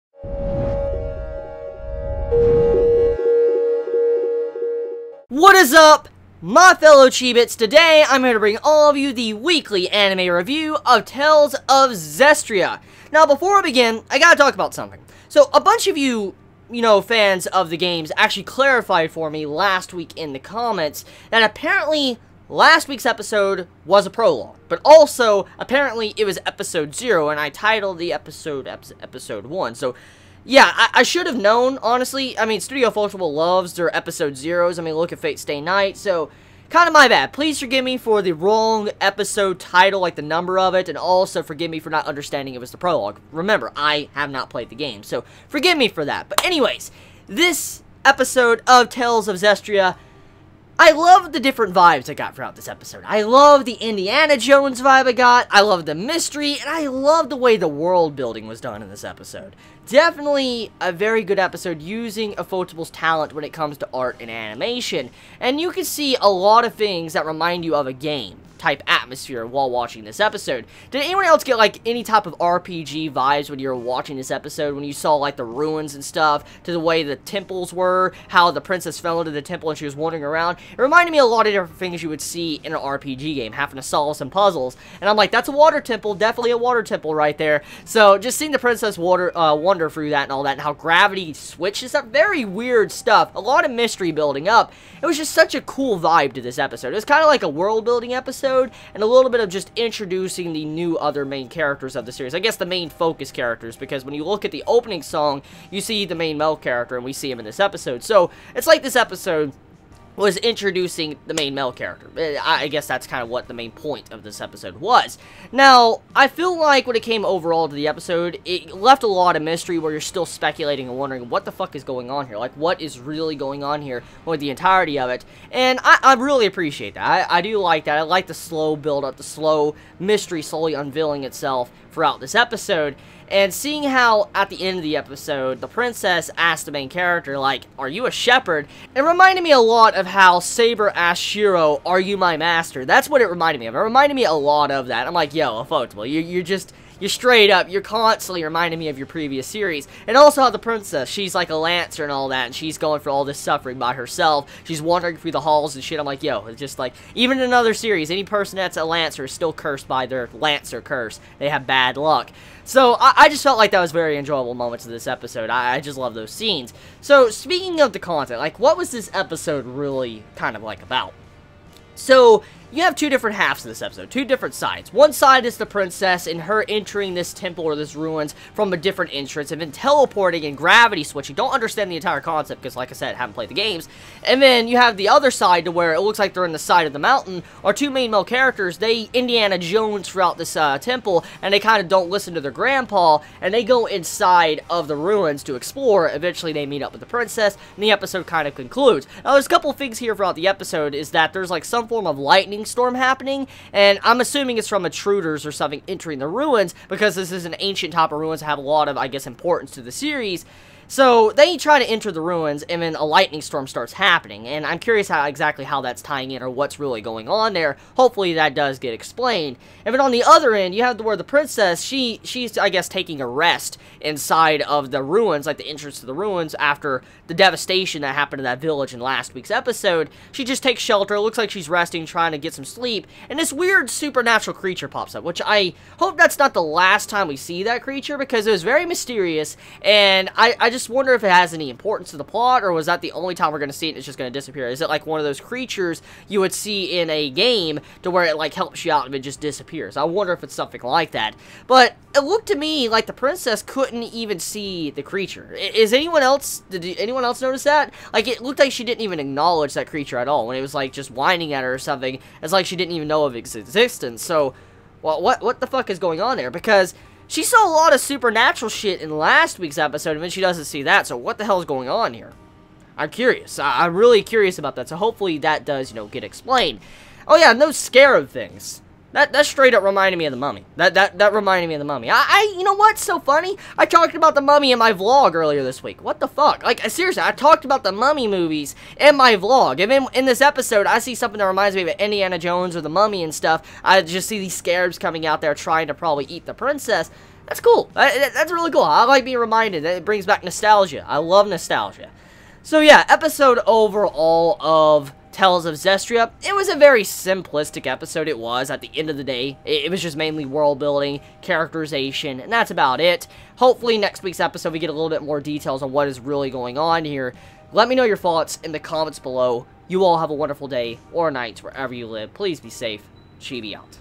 Tales of Zestiria the X Episode 1 Anime Review - Exploring Ruins
Tales of Zestiria the X Episode 1 gives us two different perspectives this week. We focus on the Princess & the two main male characters. Ready for Tales of Zestiria the X Episode 2 Review?

For any fellow Chibit that wants to send something:
2069 Johnson Chapel Road, 38583, Sparta, TN